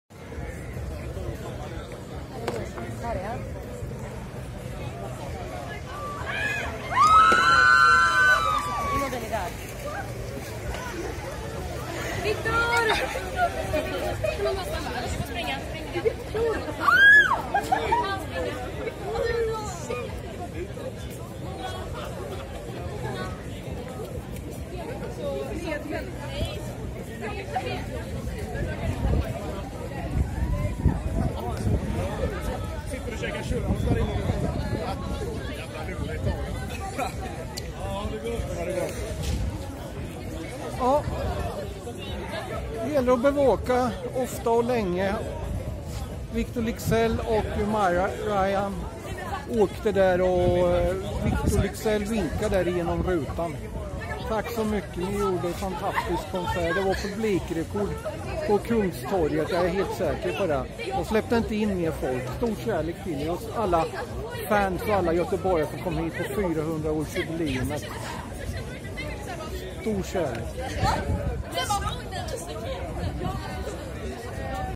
Här lämnar Miriam Bryant och Viktor Leksell superkonserten i Göteborg
Miriam Bryant avsluta Göteborgs 400 års jubileum, som har firats under 100 dagar, med en smockfull konsert på Kungstorget i Göteborg.

 Som gästartist fick vi se: "han är en av mina bästa vänner och han heter Victor Leksell, sa Miriam Bryant, och ett öronbedövande jubel uppstod från den unga publiken närmast scenen när Leksell äntrade scenen

Foto: Peter Ahlborg. Inspelat i Göteborg, Kungstorget, söndagen den 3 september 2023.